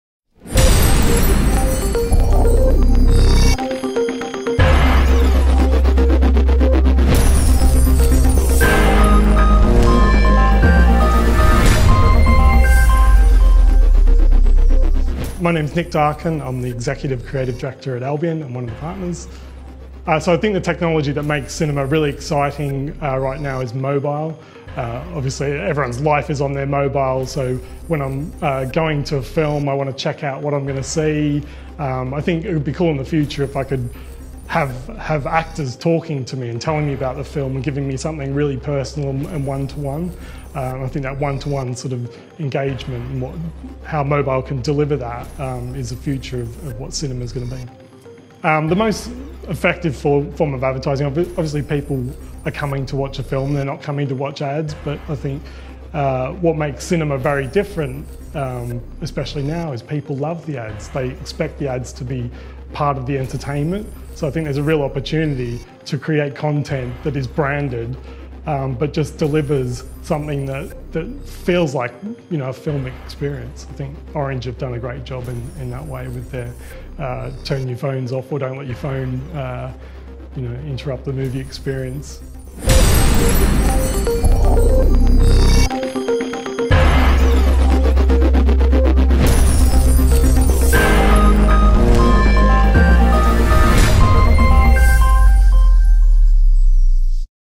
Nick Darken at Albion London on technology and innovations in cinema advertising
Digital Cinema Media asked influential business leaders how they think technologies and innovations can enhance the cinema-goers experience and what cinema ads are most effective.

Nick Darken, Executive Creative Director at Albion London